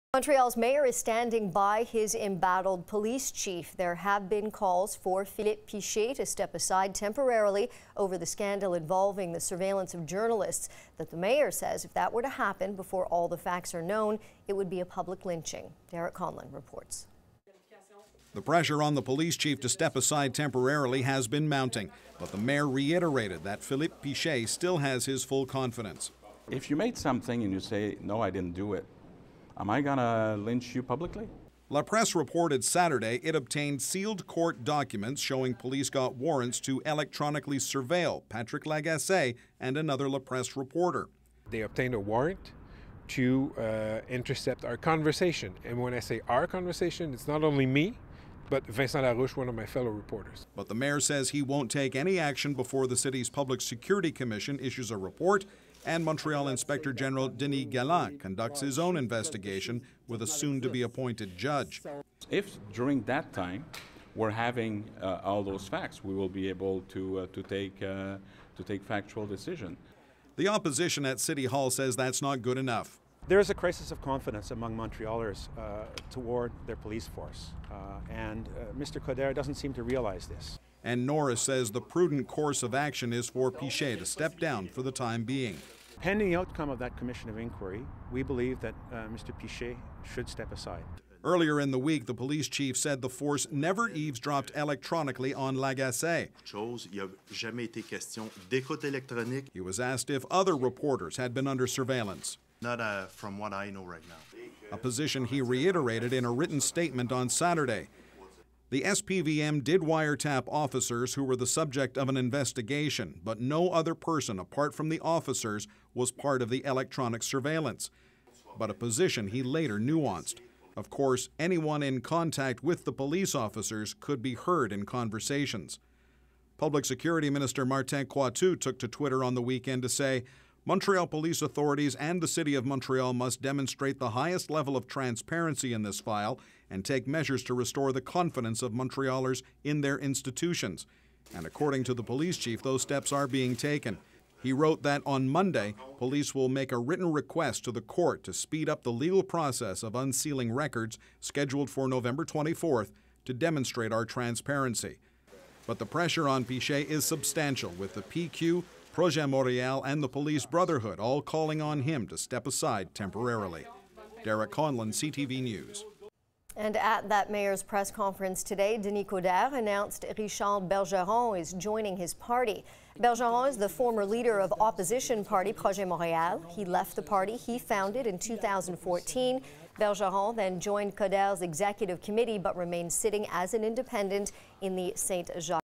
Coderre stands by police chief amid spying investigation
Montreal Mayor Denis Coderre is standing by embattled police chief Philippe Pichet.